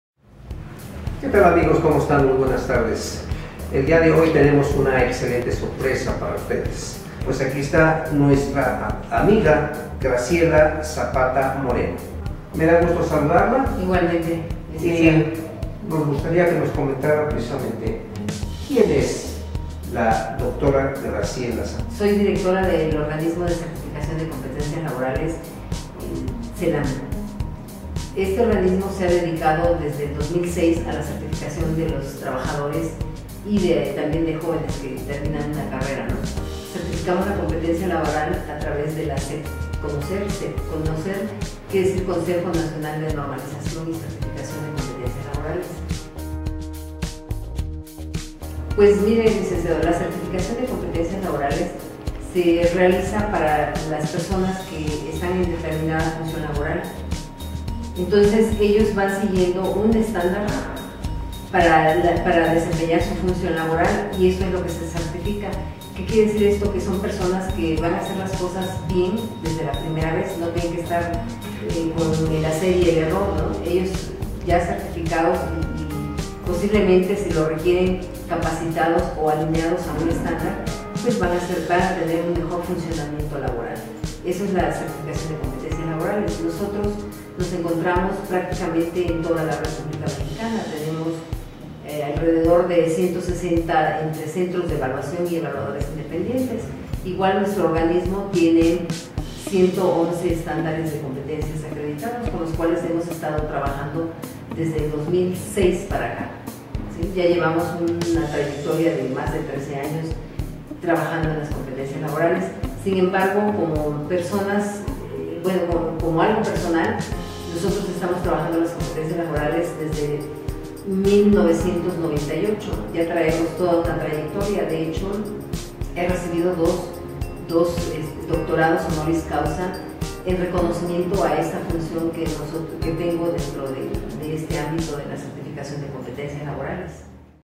1 Que es una Certificadora
La Dra Graciela Zapata Moreno le Explica a Ceferino Colin Garcia y a todo el publico que es una Certificación Laboral